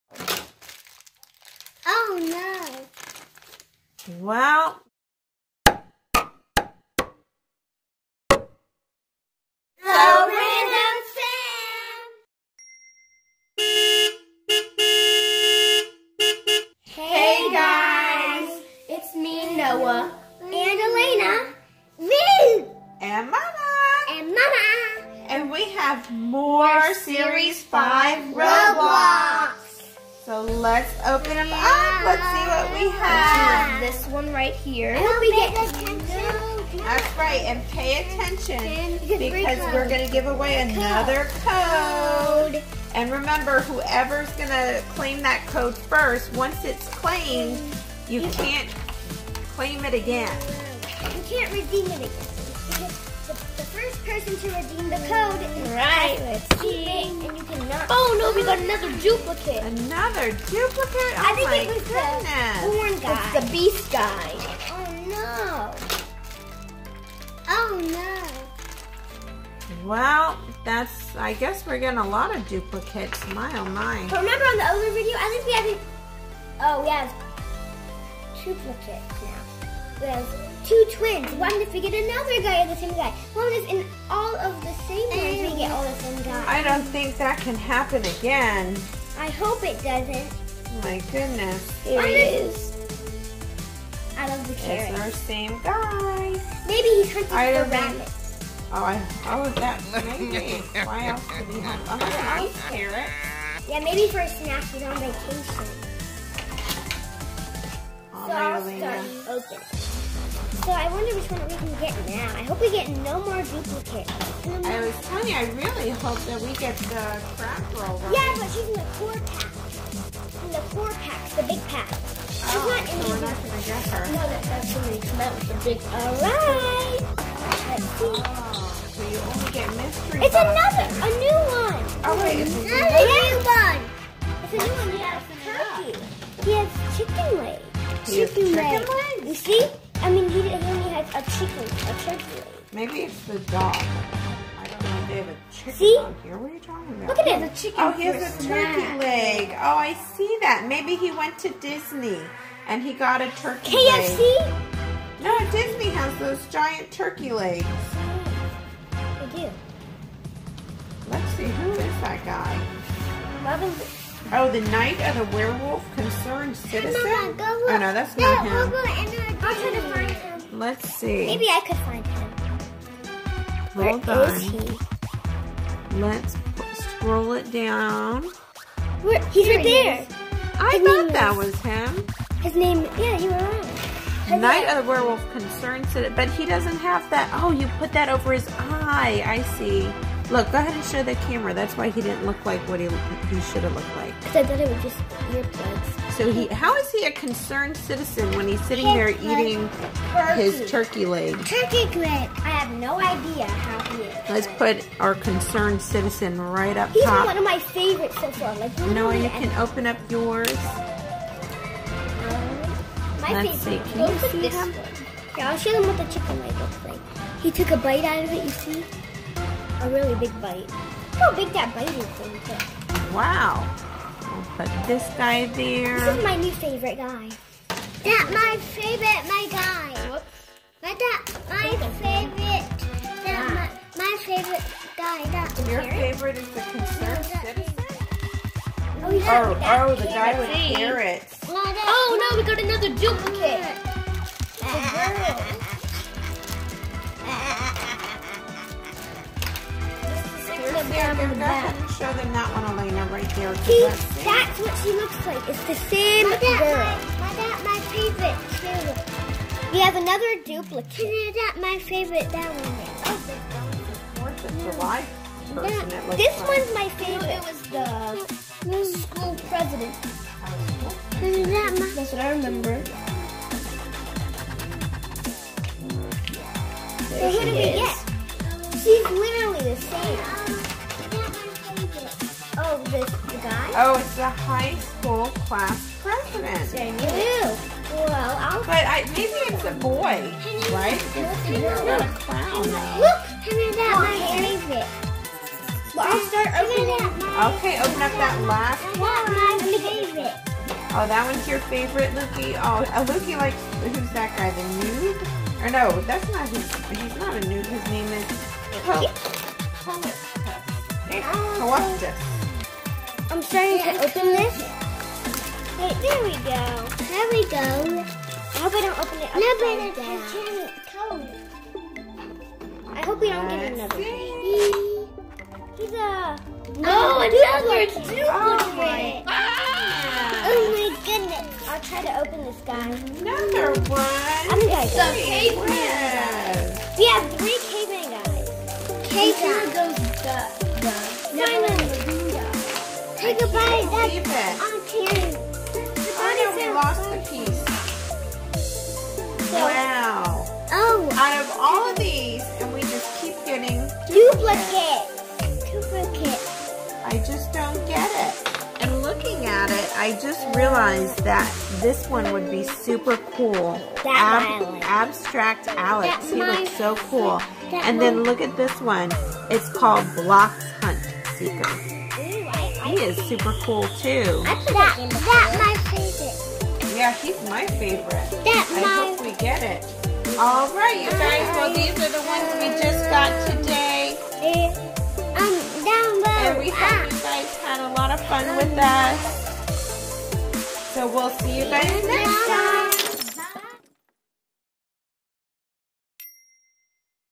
Roblox toys series 5 blind boxes "CODE GIVEAWAY" opening unboxing | SooRandomFam
Roblox toys series 5 blind boxes bonus chaser code giveaway unboxing toy mystery figures. We are opening more ROBLOX and giving 1 code away. Once the code is redeemed the code will no longer work. Let's open to see what's in side! Which series 5 ROBLOX is your favorite? Check out other Roblox videos below

ROBLOX SERIES 5 VIDEOS